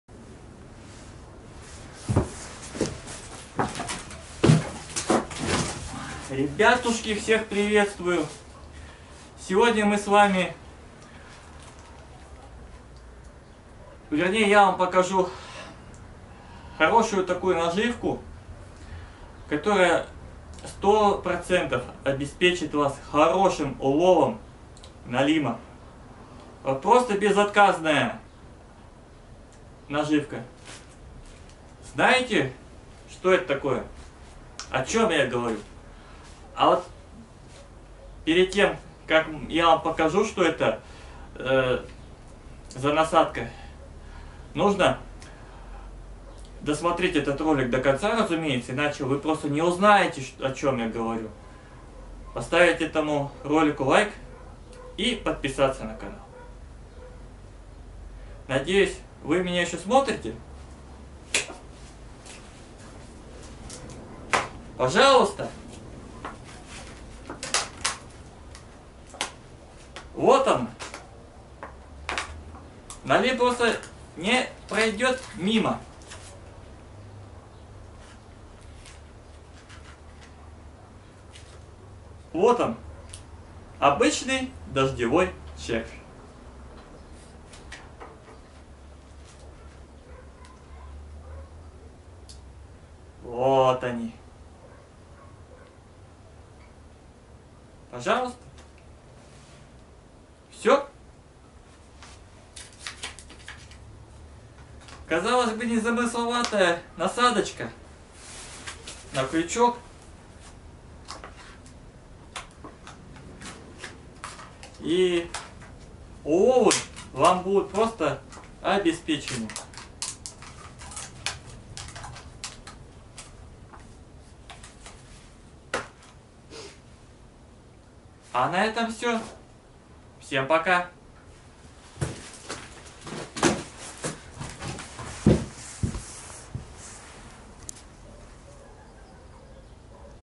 Лучшая приманка! НАЛИМ не останется равнодушным!
Обзор самой известной и уловистой приманки на налима. В ближайшее время проверим это на практике! Причём найти её можно круглый год, в том числе и зимой (зимой чаще всего в магазине), а летом можно и у себя под ногами буквально.